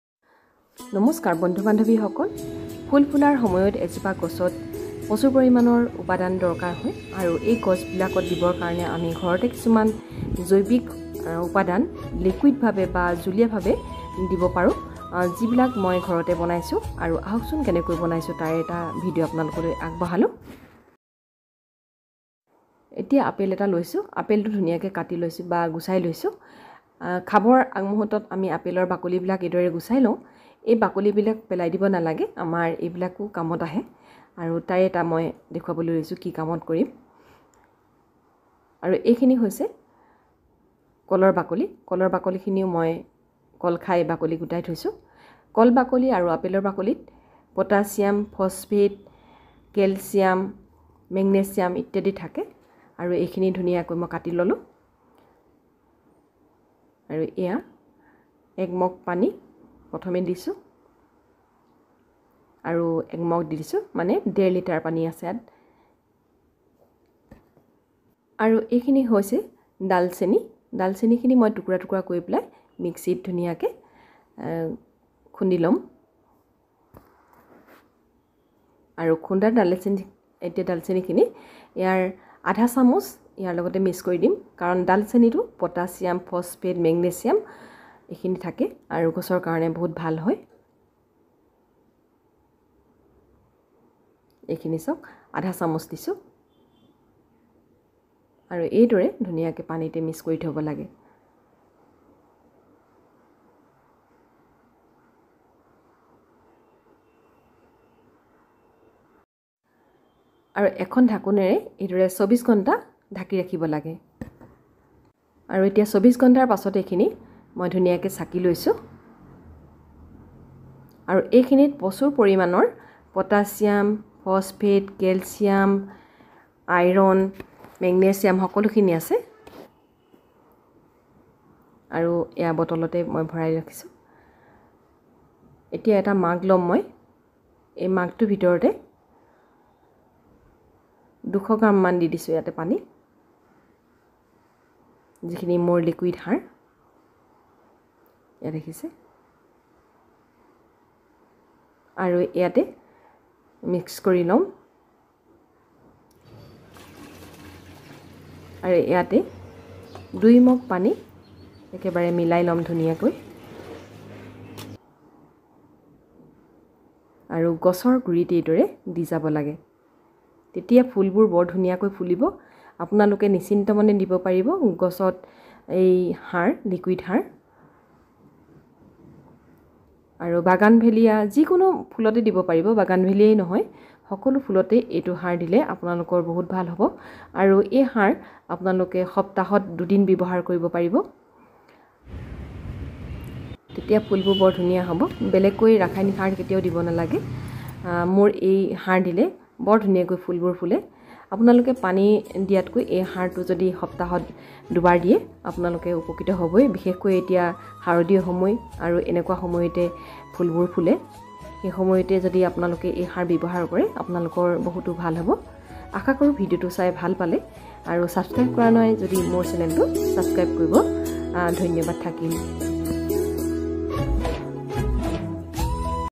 miracle liquid fertilizer for plants ঘৰতে প্ৰস্তুত কৰিব পাৰি।
apple and banana peels are rich in potassium, calcium ,phosphate etc which are essential for the plants.